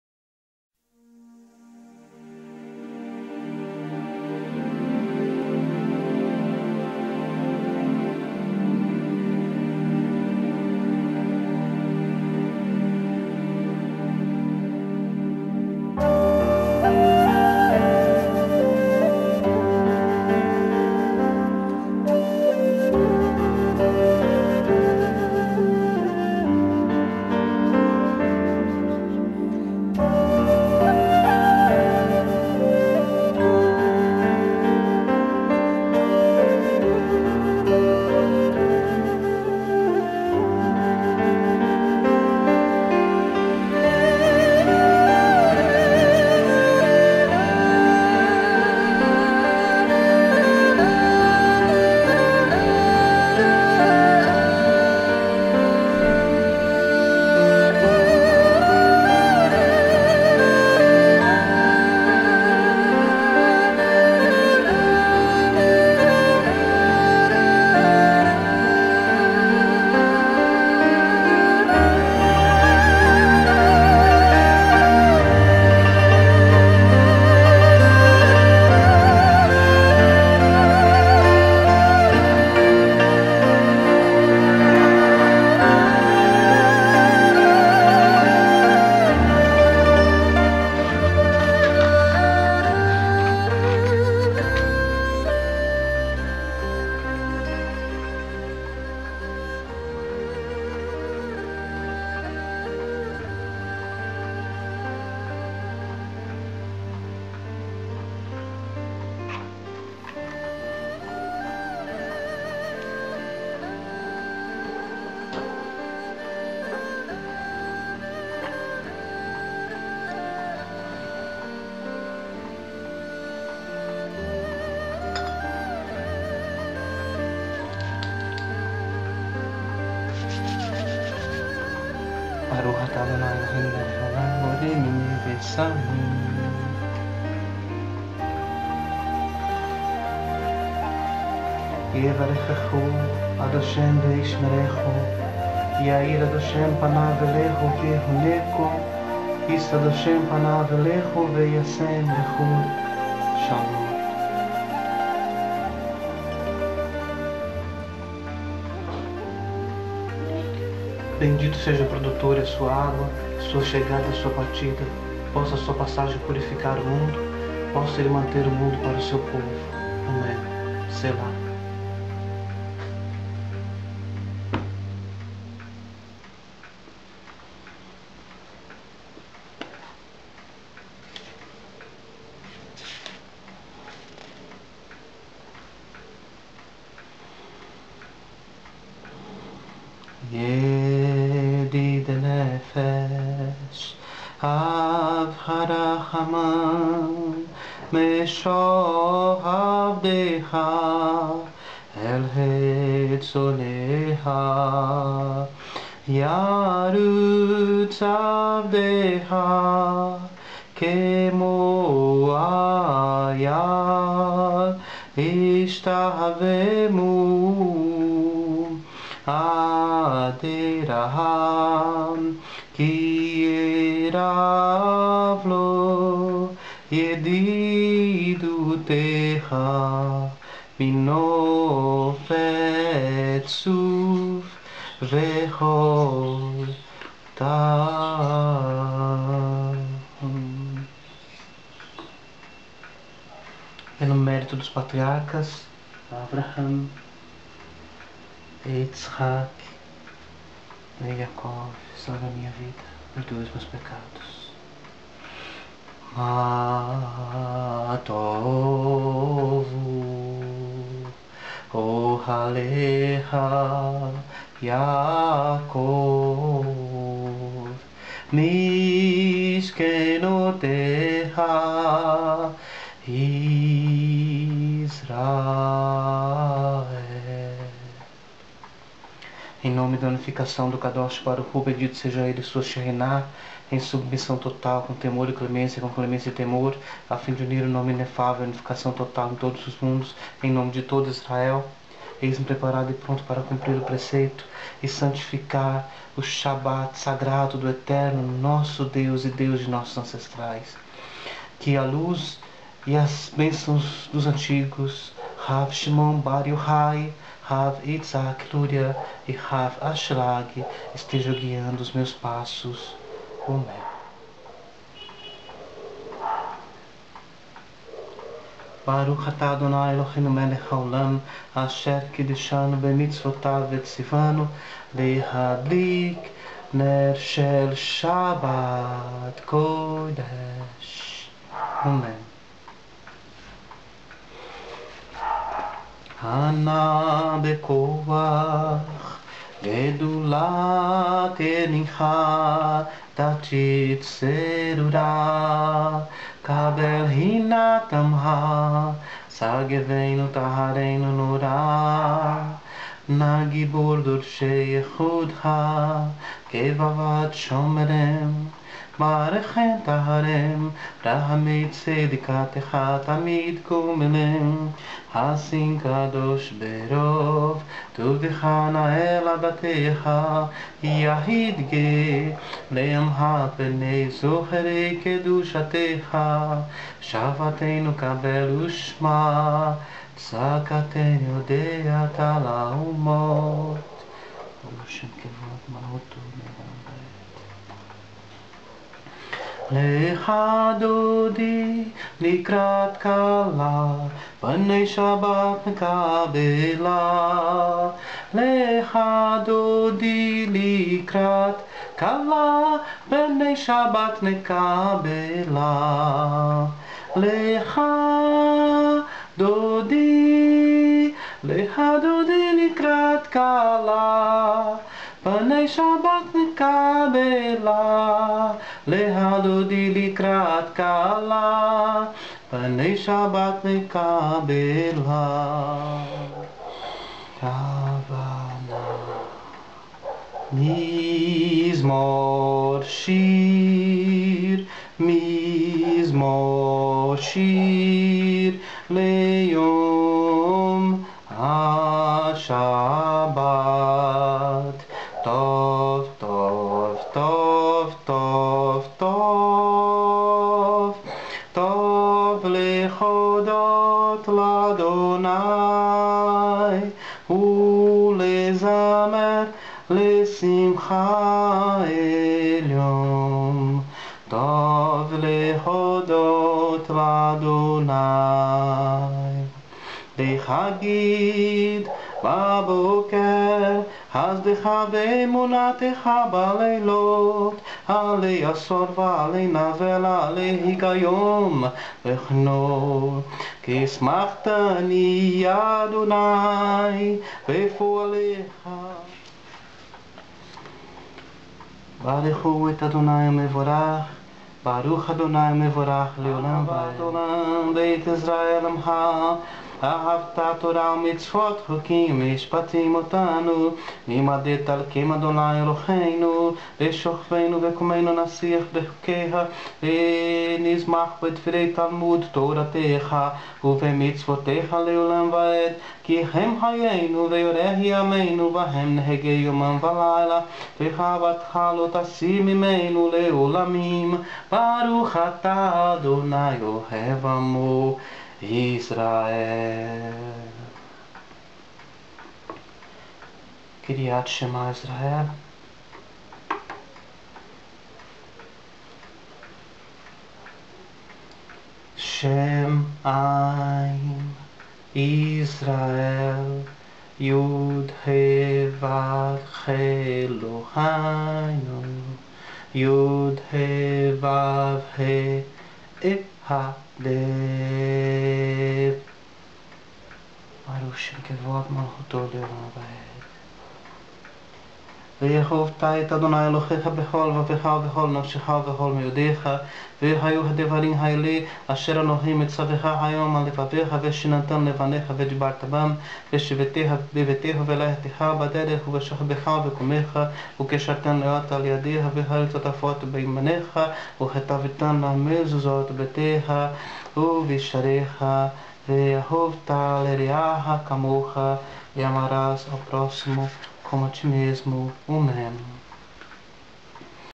Shabath (reduzido sem arvit) Iº Part
Ritual simples de shabath mas com muita kavanná e amor. Recitado por Misha"Ël Yehudá Ha"Levi em seu "chader (quarto de estudos e orações)". Contém uma homenagem aos judeus do exílio, os anussim, e suas buscas para se sentirem dignamente judeus.